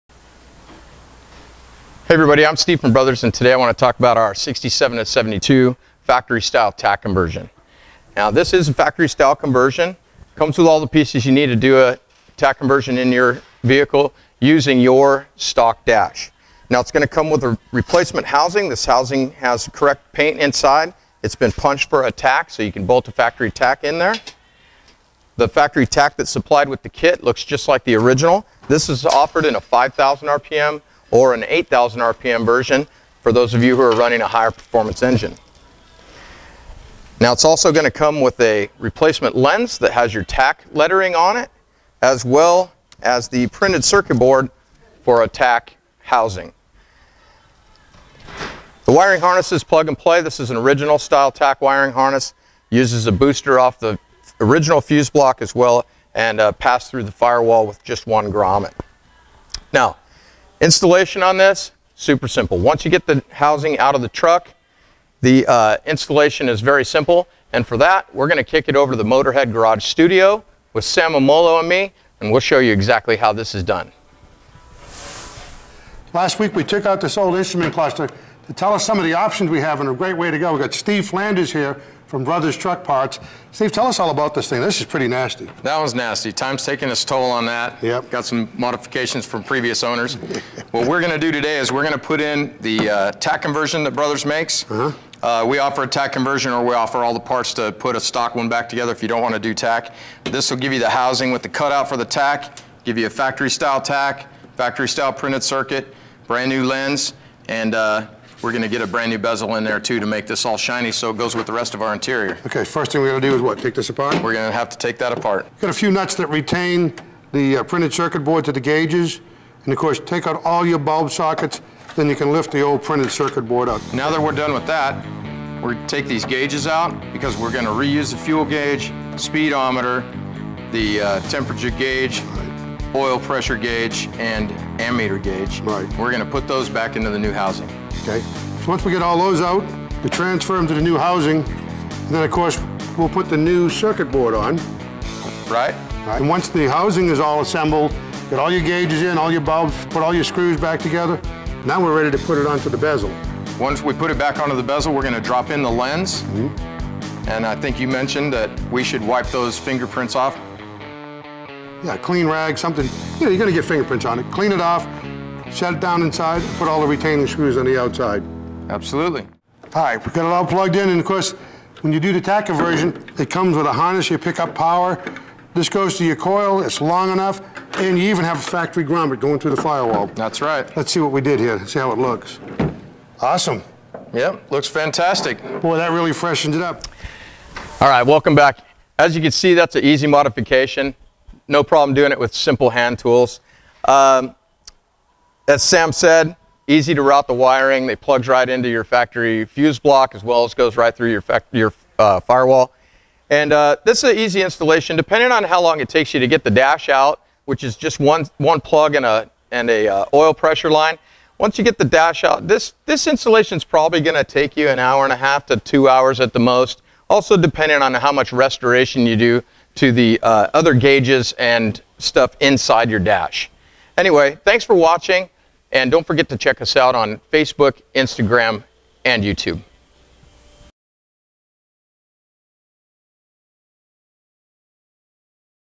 67-72 Chevy & GMC Truck Tach Conversion
BROTHERS complete Tach Conversion kit includes a reproduction tachometer, instrument cluster lens, printed circuit board, wiring and two piece metal housing for a complete conversion. Our Original Type kit installs easily into your trucks dash with common hand tools. This video talks about some of the features of this kit as well as shows how easy this conversion really is. Available with 5,000 or 8,000 RPM tachometer as well as various accessory gauge combinations.